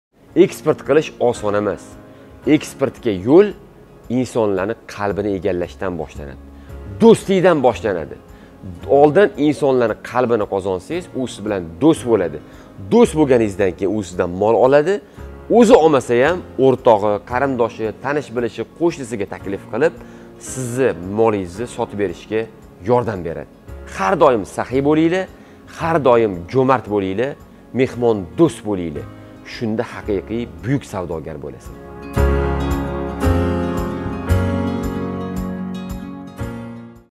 Eksportga yoʻl — nimadan boshlanadi?
Ko'pchilik eksportni shunchaki mahsulotlarni chetga chiqarib sotish jarayoni deb o'ylashadi. Ammo bu jarayongacha o'sha chet el davlatlari bozorlarini o'rganish, xalqining fikrlarini bilish kabi tahlil jarayonlari mavjud.

Ushbu video sizga manzur kelgan va foydali bo'lgan bo'lsa biz bundan xursandmiz o'zizngizni qiziqtirgan savollarni izohlarda qoldiring